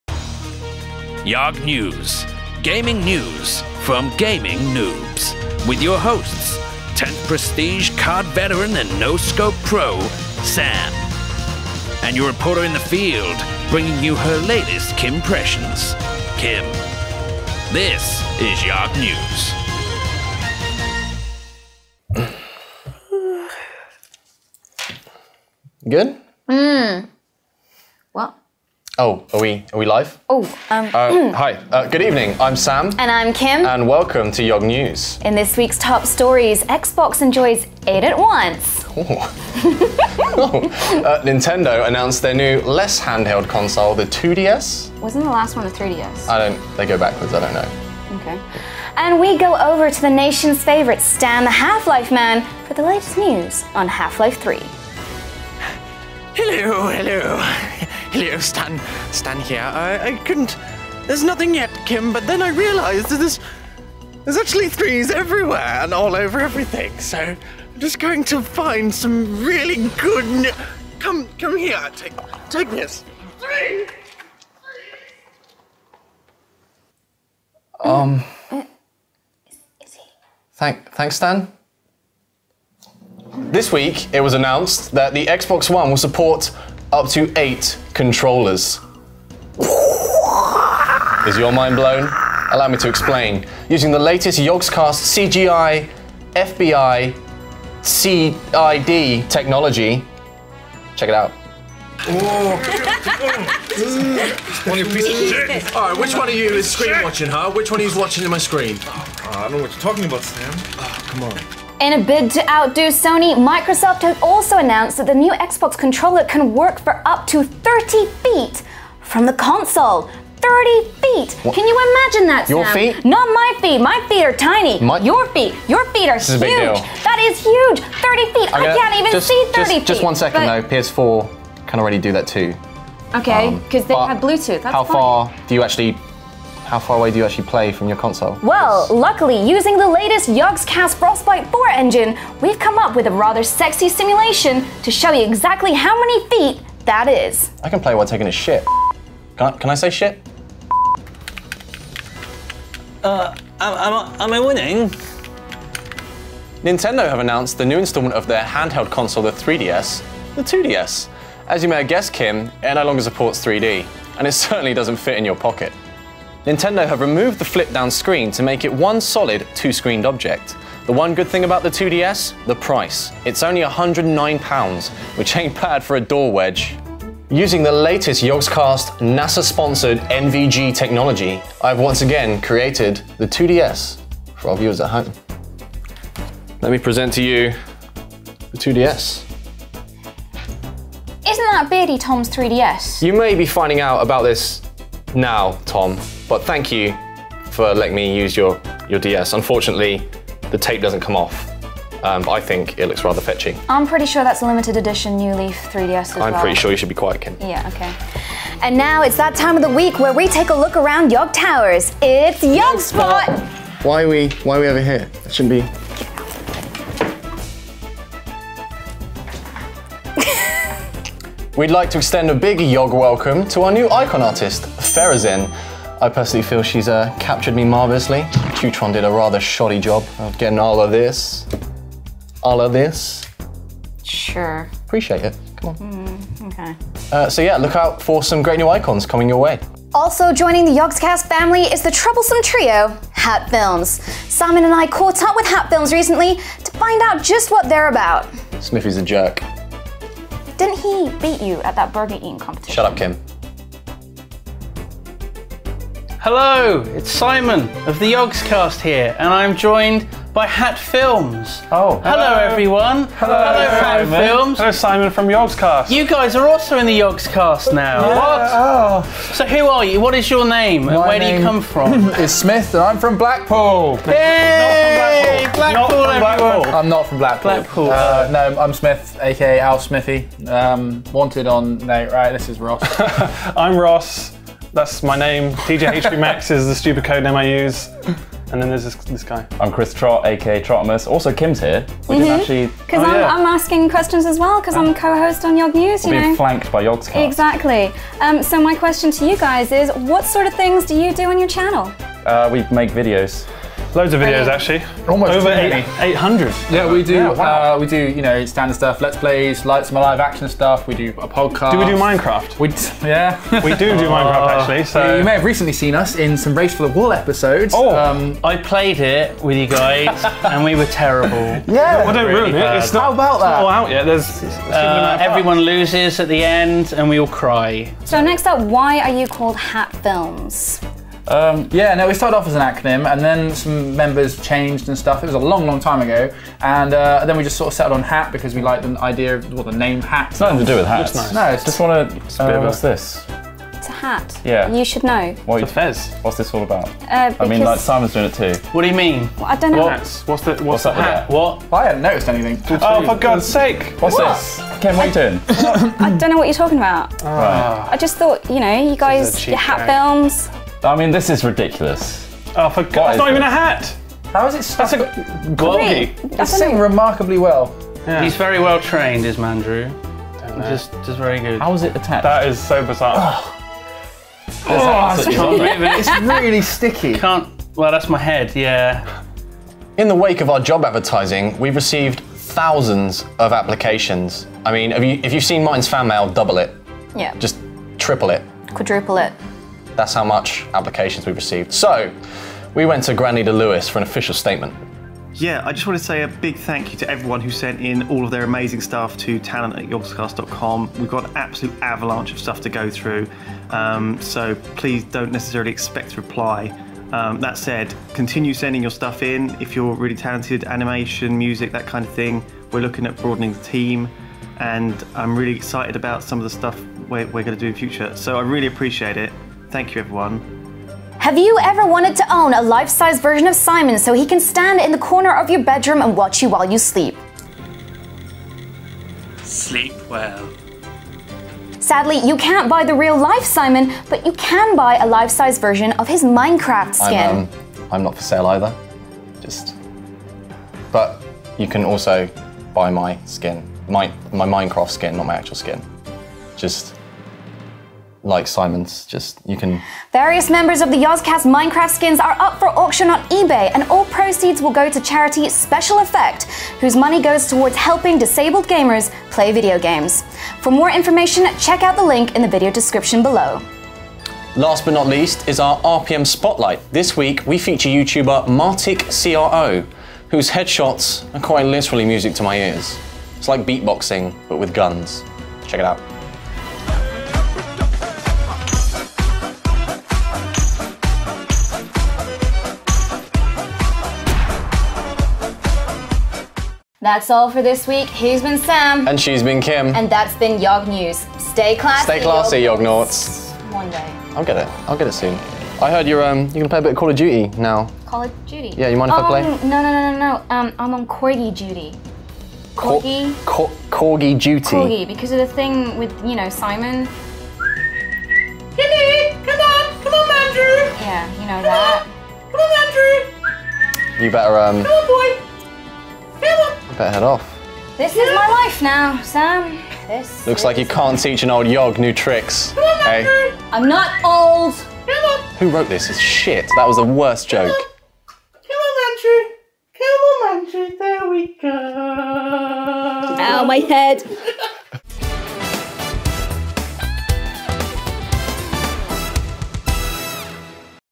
YogNews - 8 Player Xbox One, 2DS News and Skin Sale!
Welcome to YogNews! This week's top stories - Xbox One can support up to 8 players, Nintendo reveal the 2DS, and we welcome Hat Films! Plus, our skins are up for sale! Links to all the top stories below!